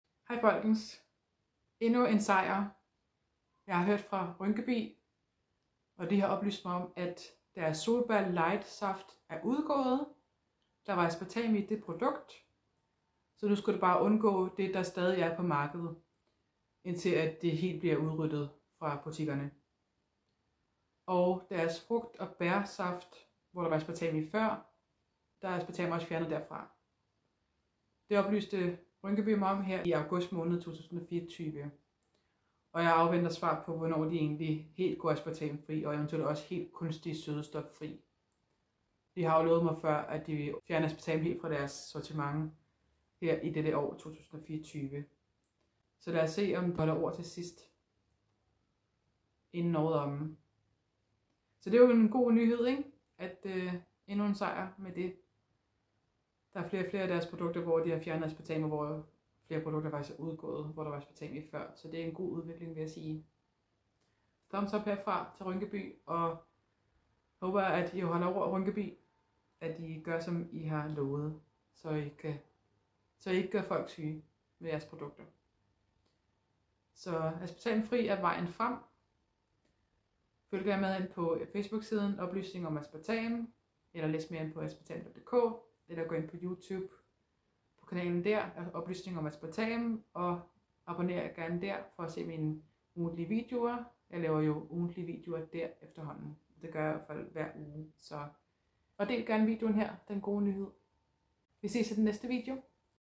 Endnu en sejr! Tiltag fra Rynkeby 🥳 Another victory! Initiative from Rynkeby (danish foodsupplier)
Endnu en sejr! Tiltag fra Rynkeby 🥳
Så er Rynkebys solbær light saft udgået og deres frugt og bær saft er nu Aspartam-fri, det har Rynkeby oplyst mig om i august 2024.

Se også min video om hvad Rynkeby ellers ønsker at gøre ift. Aspartami deres produkter